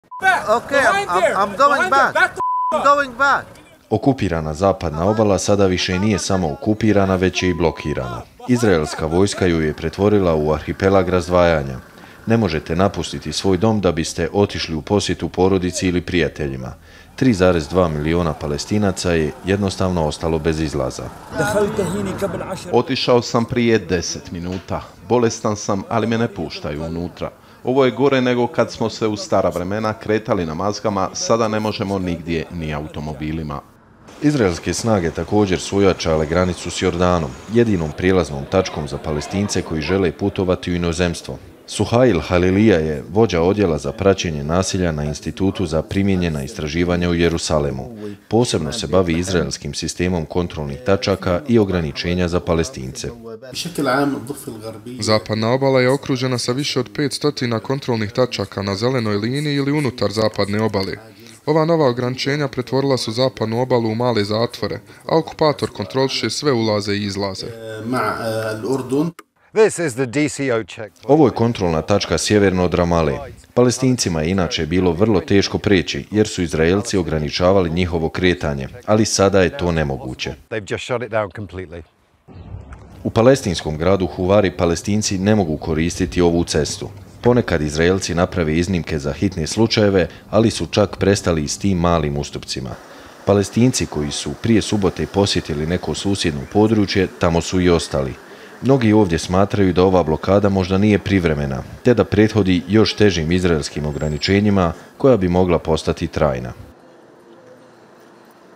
Zapadna obala u blokadi, Izraelci ograničili kretanje Palestincima
Izraelska policija u okupiranom istočnom Jerusalemu saopštila je da je ubila dvojicu Palestinaca koji su ih u utorak gađali kamenjem.

Izrael je također blokirao puteve unutar okupirane Zapadne obale.